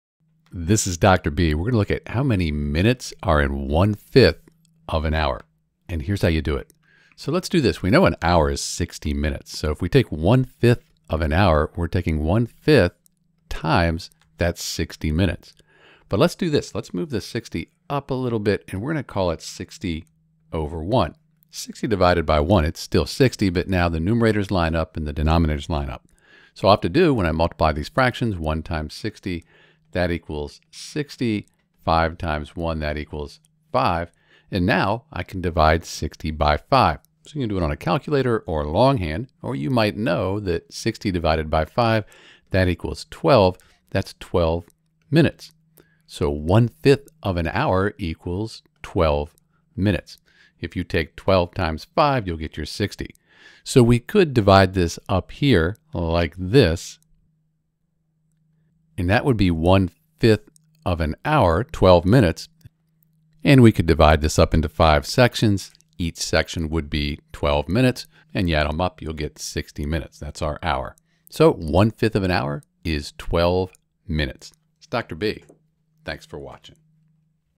How many minutes in 1/5 of an hour?
To find out how many minutes are in 1/5 of an hour, recognize that one hour is equivalent to 60 minutes. Multiply 1/5 by 60:

Minutes = (1/5) * 60

Therefore, when we multiply, we get minutes as a fraction.

Minutes = (1 * 60) / 5 = 60/5

We then simplify the fraction:

Minutes = 60 / 5 can be simplified to 12 minutes.

Therefore, 1/5 of an hour is equal to 12 minutes.

You can use this technique to find the number of minutes in any fraction of an hour.